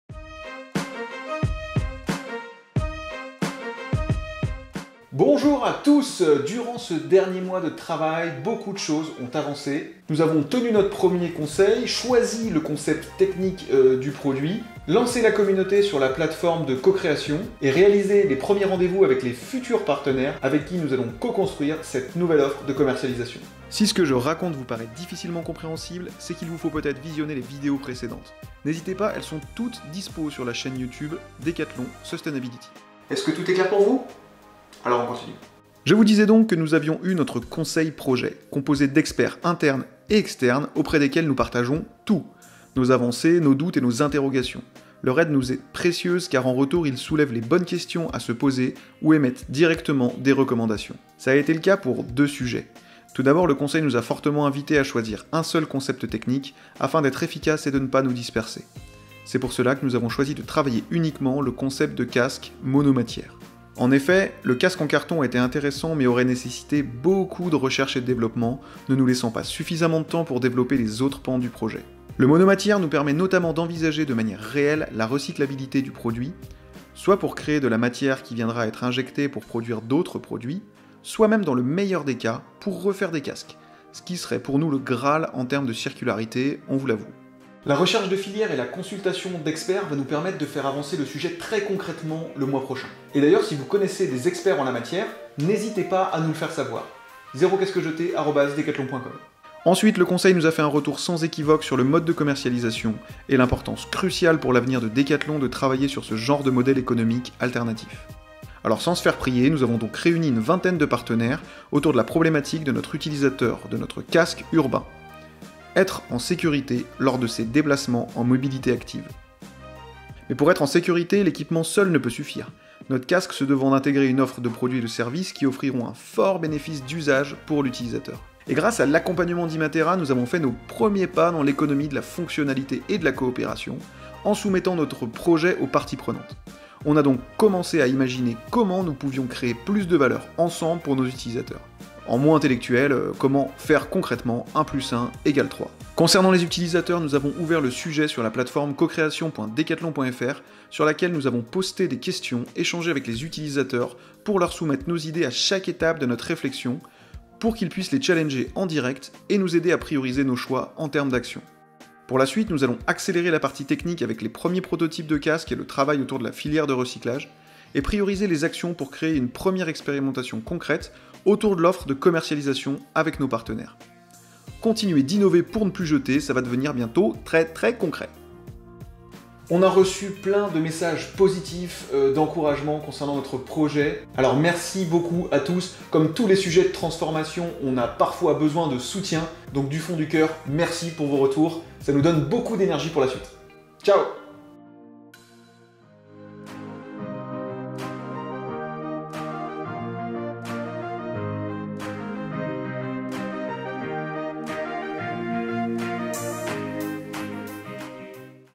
Projet ZéroCasqueJeté : ON CONTINUE !
Débrief mensuel #2 de notre travail avec beaucoup d'avancées en termes de décision sur le produit et nos premiers pas concrets dans l'économie de la fonctionnalité...